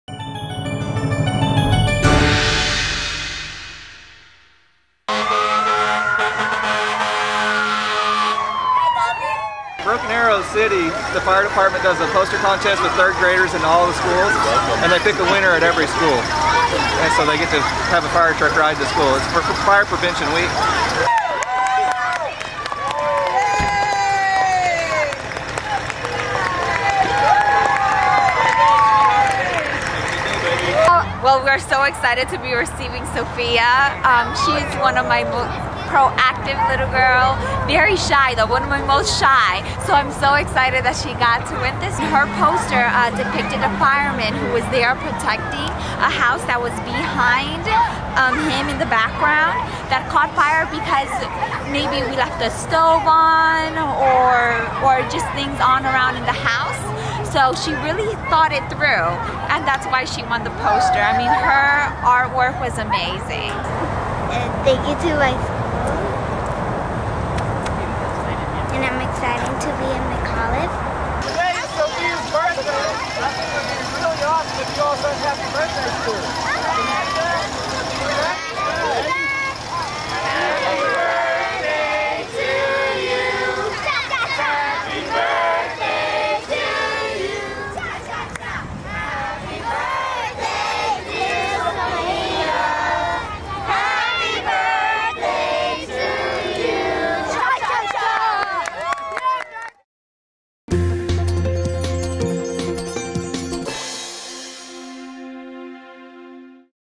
McAuliffe third grader takes fire truck to school
Broken Arrow firefighters held their annual fire safety and prevention poster contest recently. Each third grade class within the Broken Arrow city limits qualifies to compete, and one winner from each receives a ride to school on a fire truck. In the Union School District, Sofia Lopez is the proud winner from McAuliffe Elementary and received her ride to school on her birthday on November 7, 2013.

For more photographs and information, go to this link on the Union Schools'